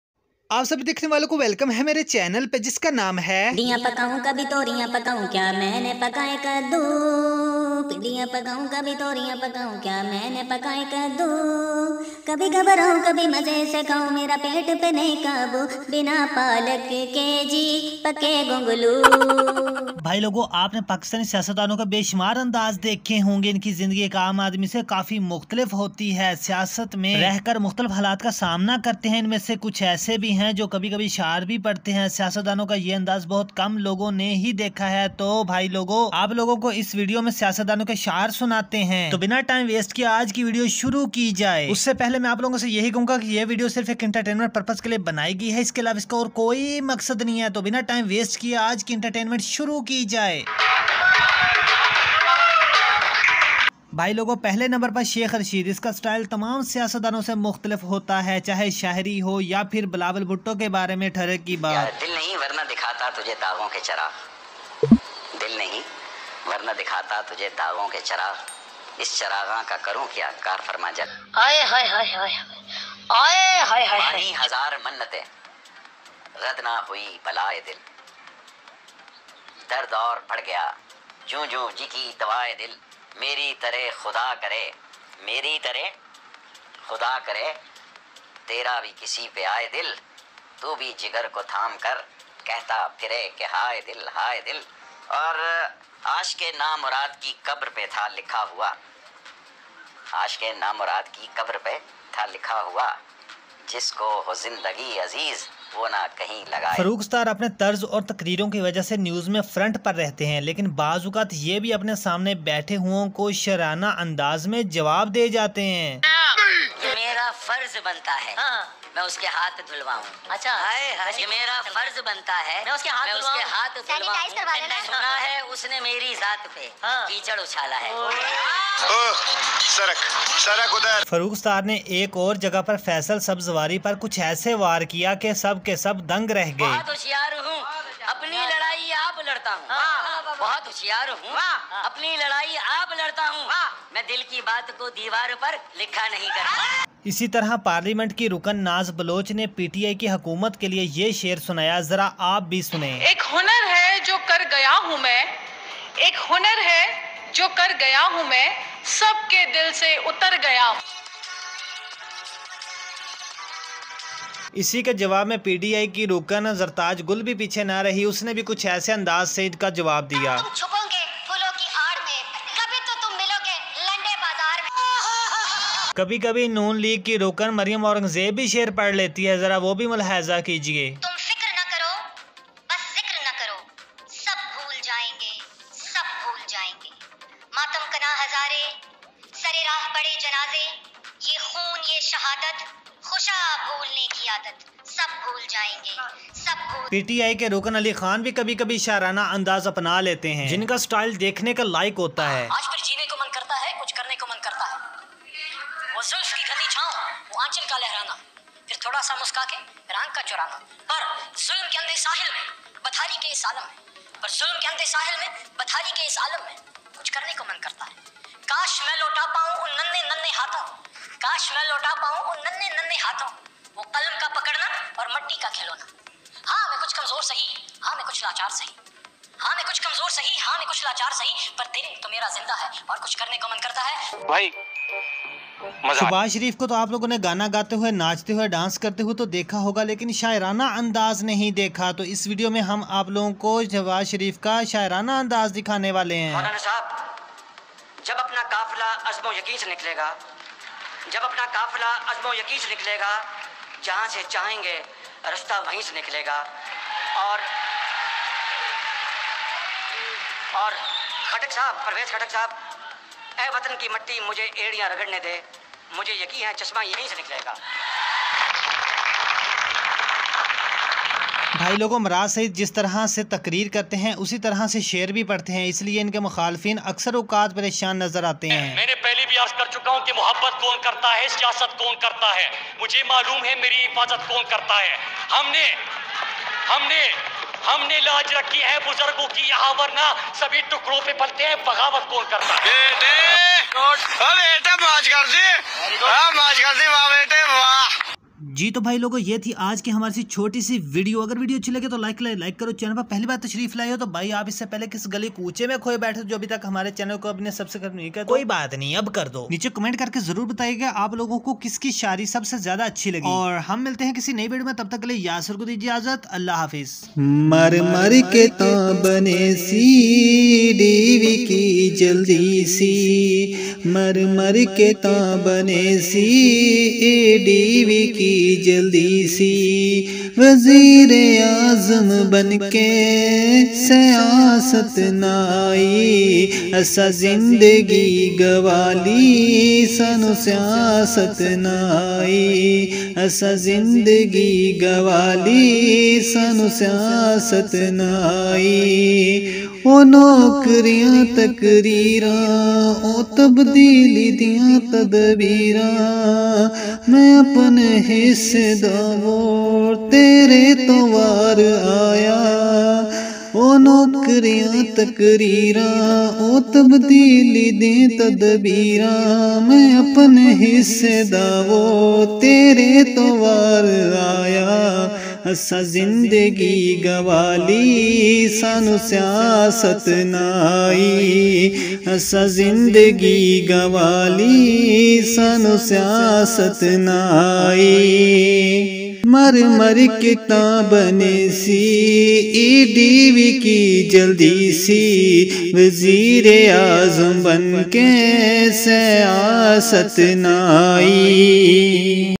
Best Poetry lines by Pakistani Politicians|Pakistani Politicians Best Poetry Expose|Yasir Sabziwala
In this video we will talk about Pakistani Politicians and their poetry taste. we will show you Sheikh Rashid , Satttar, Mariam orangzaib and pti workers.
in ma sa kuch ashaar mazahia andaz ma hy.